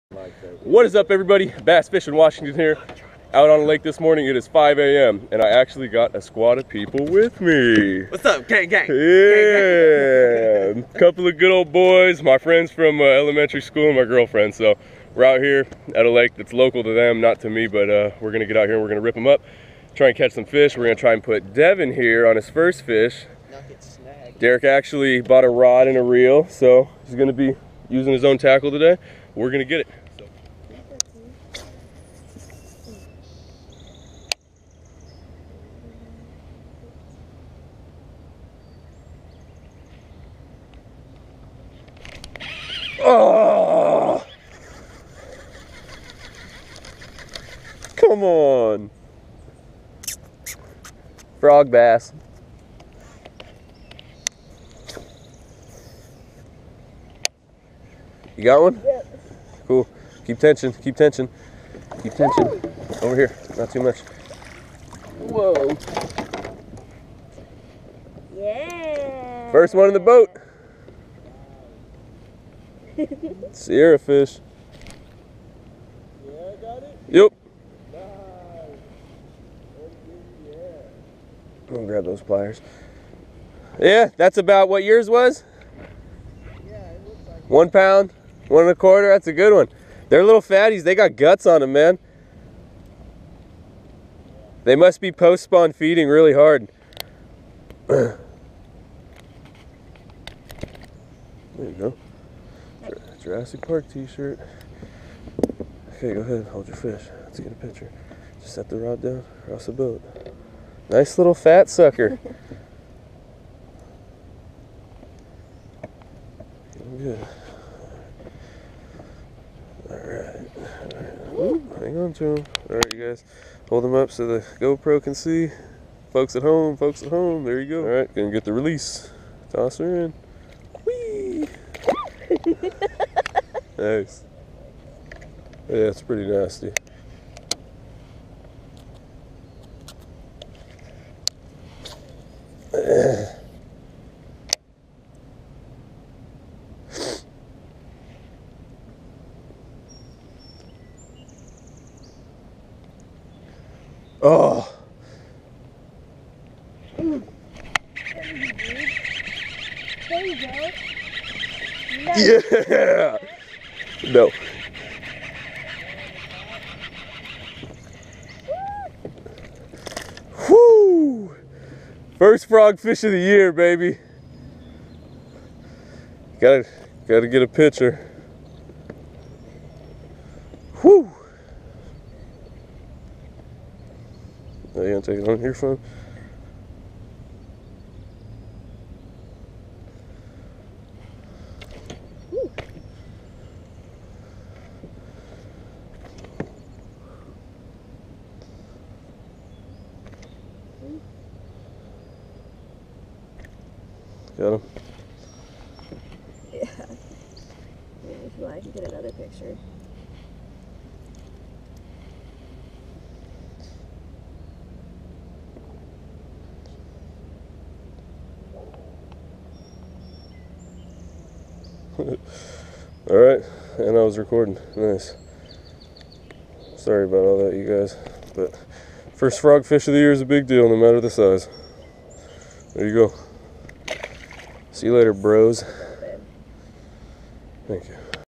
First Frog Fish of The Year!
FISH ON! I have been waiting such a long while to finally get a frog fish! And I know, its not huge, it aint no beast or behemoth. But its my first frog fish of the year and I will gladly take any size. Now that the topwater bite is in the midst of the best time to be using topwater ill definitely be getting sick shots of more frog hook sets and blowups!

Technique: Throwing the frog near the bank if not on the bank. Hopping the frog into the water, Letting the ripples dissipate then working it back with pops and a walking the dog technique. Slick calm water was great for frogging on pads and matts.

Weather: AMAZING start to the day with nice 60 degree weather in the morning and no wind. The sun slowly crept over the tree line at about 12 and finally hit us. By then we were heading off the water.

Gear Used:
-7'0 Medium Fast Action St. Croix Mojo Bass
-Abu Garcia ProMax 7.1.1
-50LB Powerpro Braid.
-Spro Popping Frog

Camera Gear:
-GoPro Hero+
-Chest Mount
-Gooseneck Attachment
-San Disk 32 gig SD Card

You guys are the driving force behind me making these fun videos! PEACE!